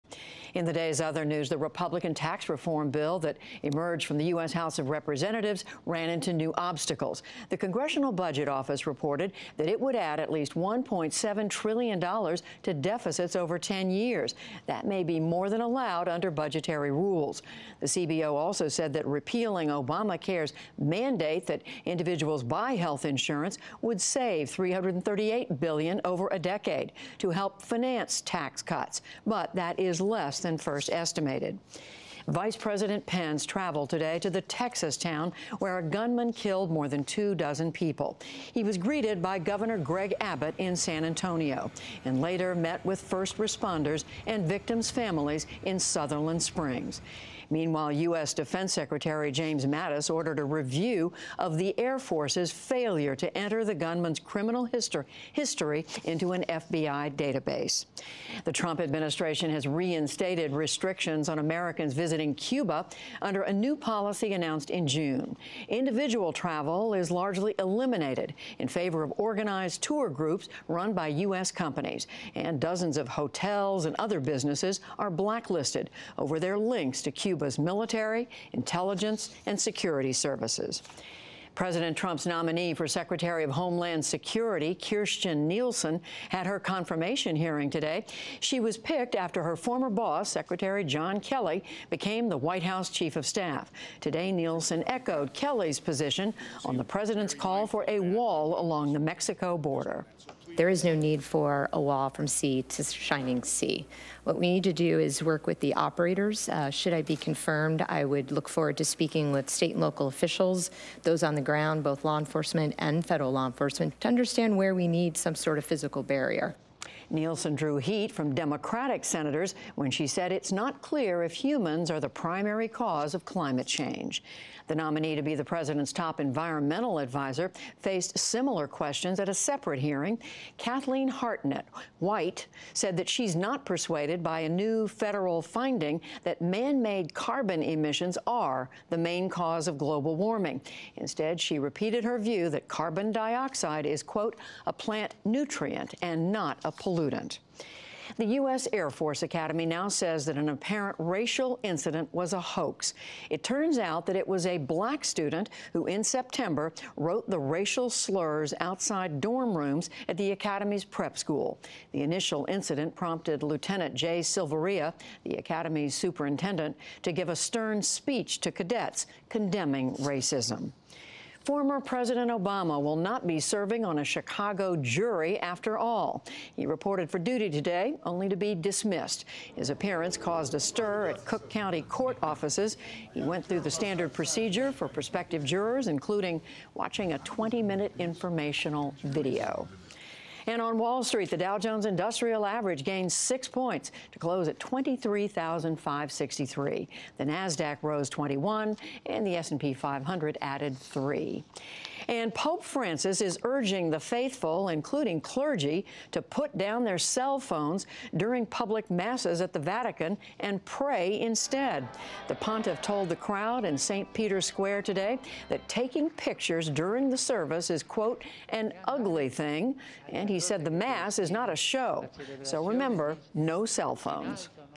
News Wrap: GOP tax bill runs into roadblocks over deficit estimate
In our news wrap Wednesday, the Republican tax reform bill that emerged from the U.S. House of Representatives ran into new obstacles as the Congressional Budget Office reported it would add at least $1.7 trillion to deficits over 10 years. Also, Vice President Mike Pence traveled to the Texas town where a gunman killed 26 people, meeting with first responders and victims' families.